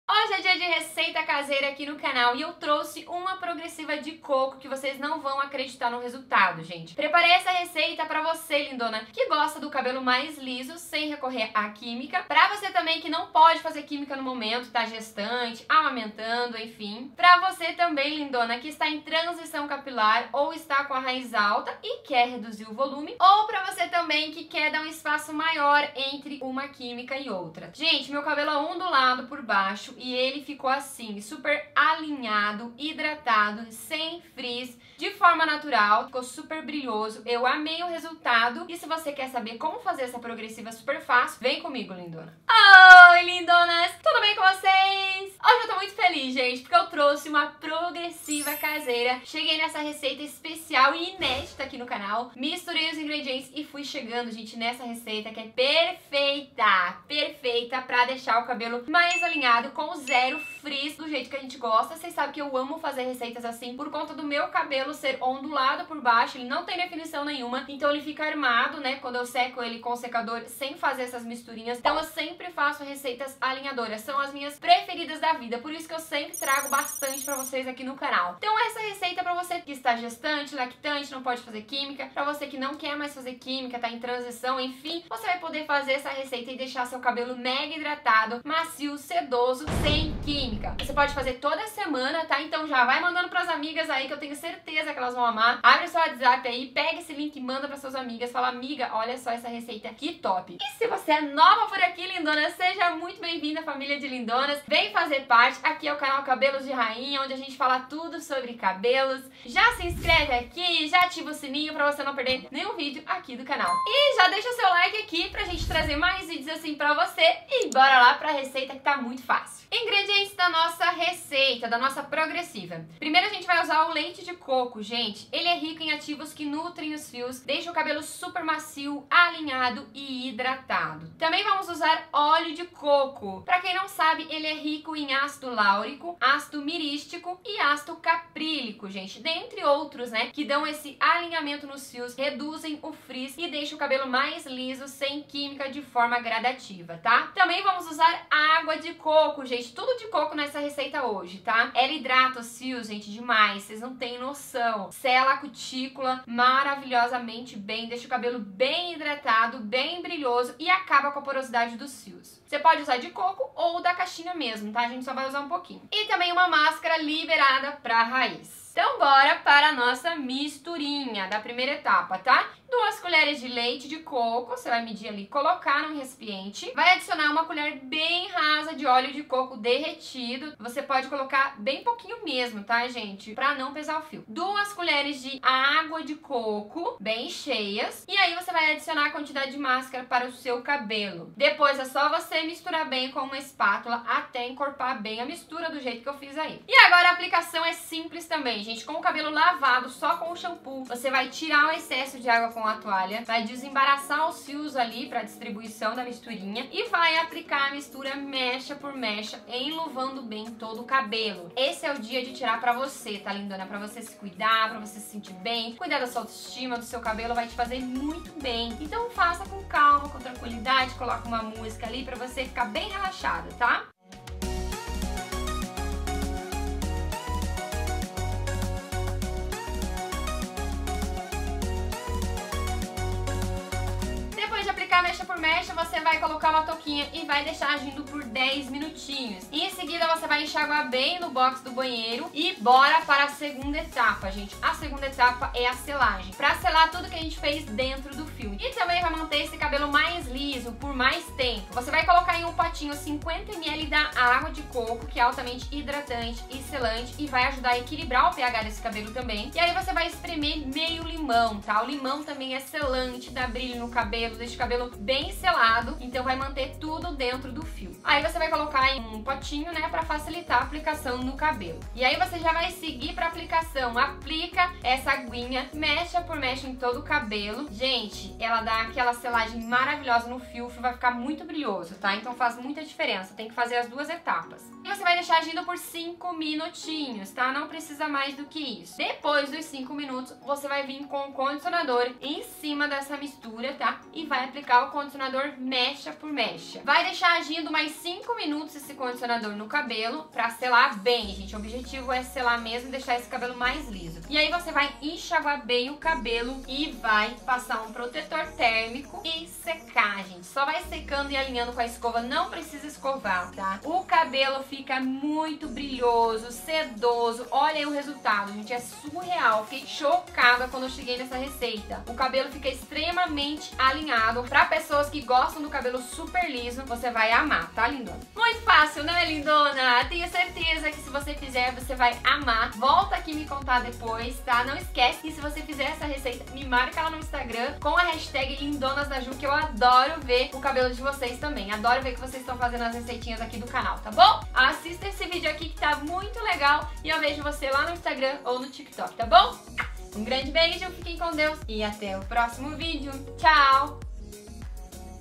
Progressiva caseira de Coco 100% natural cabelo liso escorrido e espelhado SEM CHAPINHA SEM QUÍMICA
Essa receita vai deixar seu CABELO LISO (COMO NUNCA). Teste e comprove!

Oi lindonas,
mais um vídeo no canal, espero que gostem!

Você gosta de Cabelos Mais liso Sem Química?

Gostaria de Reduzir o Volume do Cabelo e Acabar com o Frizz?

Quer saber como deixar a Raiz Lisa sem química?

No vídeo de hoje vou ensinar como fazer uma progressiva Caseira, um alisamento natural sem química.

Seu cabelo vai ficar mais alinhado e zero frizz.

Progressiva caseira de açúcar 100% natural cabelo liso escorrido e espelhado

Essa receita caseira desmaia o cabelo, derrete os fios, deixando seu cabelo mega hidratado, macio sedoso e sem química.

Você pode fazer essa mistura caseira toda semana, então lindona indica para as amigas, elas vão amar!

Perfeita para Cabelos Danificados, Ressecados, Porosas, secos e quebradiços.

Para fazer essa progressiva caseira você vai precisar.

Leite de coco - Rico em ativos que nutrem os fios, deixando o seu cabelo liso, sedoso e brilhoso rapidamente.

Óleo de Coco - Rico em ácido laurico, que alinham os fios, deixam o cabelo mais liso sem química de forma gradativa.

Água de Coco - Hidrata os fios e sela a cutícula deixando o cabelo hidratado, acabando com a porosidade dos fios.

E por último, uma Mascara liberada para a raíz.

Para saber as quantidades de cada ingrediente assista ao vídeo completo.

Deixe essa misturinha agindo por 10 minutos.

Essa Progressiva caseira alisa até CABELO CRESPO sem QUÍMICA, mas claro gradativamente.

Quanto mais você usa mais liso seu cabelo fica.

Depois que aplicar essa misturinha, vamos para a segunda receita que sela o fio.

- 50 ml de água de coco;
- 1/2 limão.

Essa segunda receita vai selar todos os nutrientes dentro do fio, dando ainda mais brilho.

Ameiii essa misturinha.

Essa receita vai deixar a RAIZ DO SEU CABELO BAIXA SEM ALISAR  é uma PROGRESSIVA CASEIRA ALINHADORA!

Topppp demais!

Teste ai lindonas e depois volte aqui me contar o que você achouuuuu!

Combinado?

Att,
Julia Doorman
Cabelos de Rainha